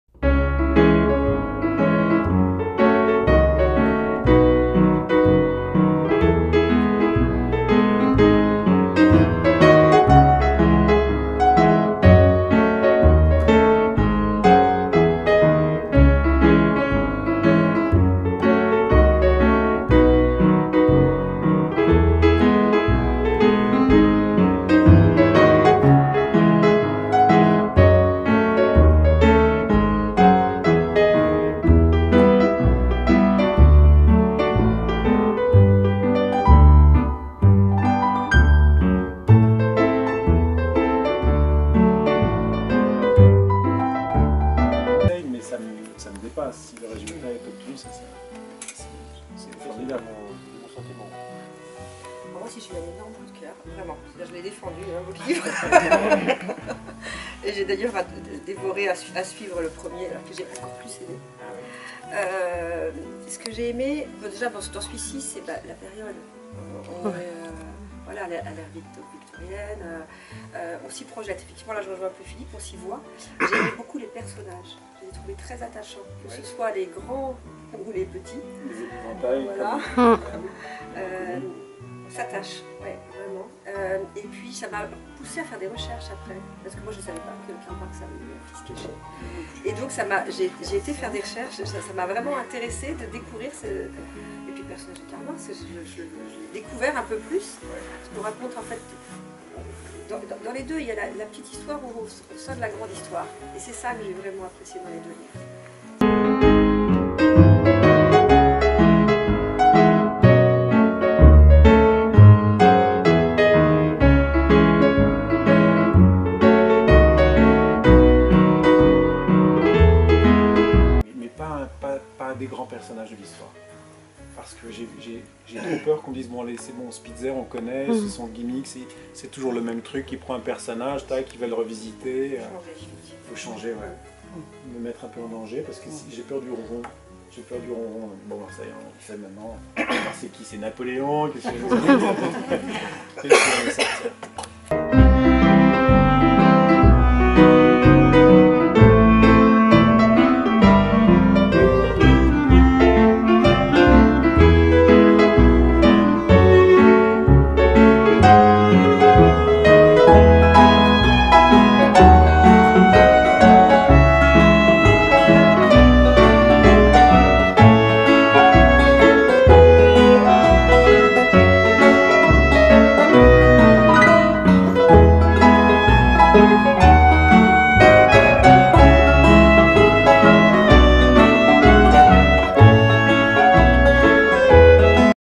Retour en images d'une belle rencontre avec Sébastien Spitzer
Sébastien Spitzer, lauréat du Prix Goncourt des Fougères 2019, est venu à la rencontre des lecteurs de la médiathèque de La Chapelle des Fougeretz.
Lien des livres sur notre catalogue :
(Le cœur battant du monde)